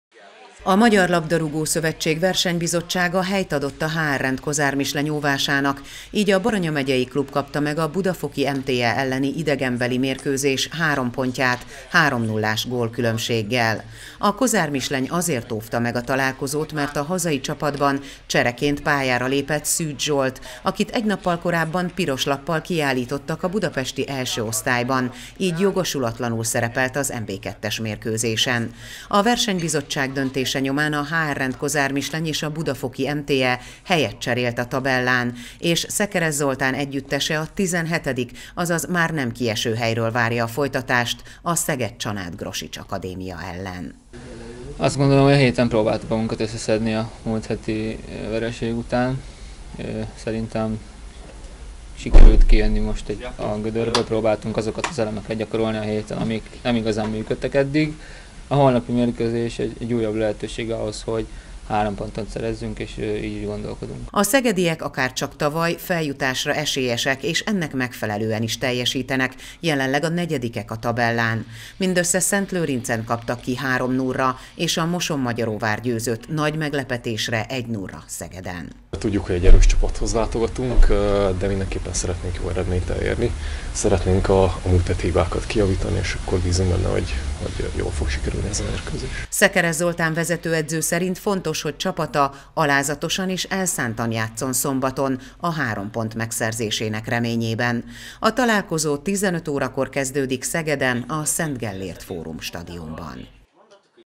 Helyt adtak az óvásnak - Három ponttal lett gazdagabb a HR-Rent Kozármisleny labdarúgócsapata
A versenybizottság ugyanis helyt adott a baranyai csapat óvásának, így a HR-Rent Kozármisleny és a Budafoki MTE helyet cserélt a tabellán, és Szekeres Zoltán együttese a 17-dik, már nem kieső, helyről várja a folytatást.